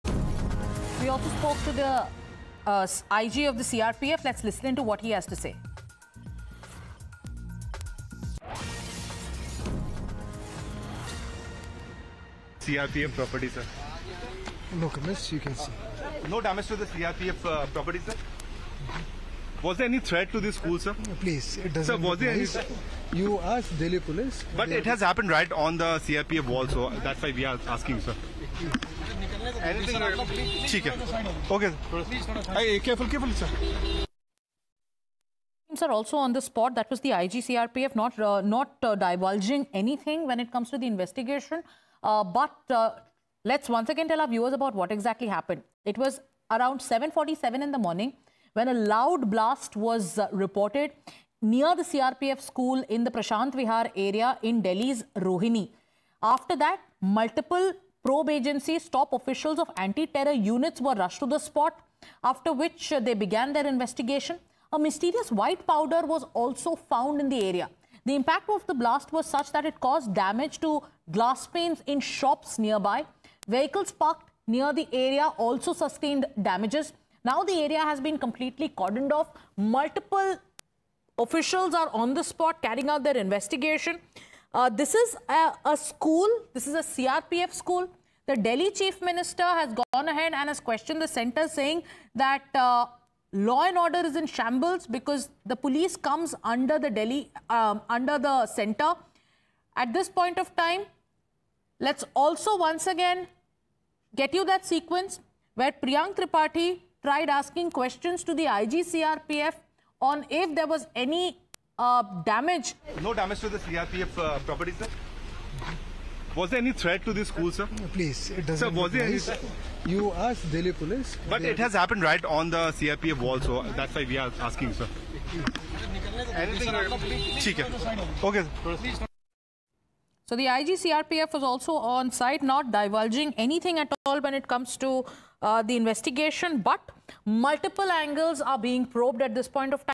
Times Now Speaks To IG-CRPF Amid Mysterious Loud Blast In Delhi's Rohini Near CRPF School | Breaking
Breaking News: Times Now spoken to the Inspector General (IG) of CRPF amidst the the incident of mysterious blast reported near the CRPF school In Delhi's Prashant Vihar Area Rohini.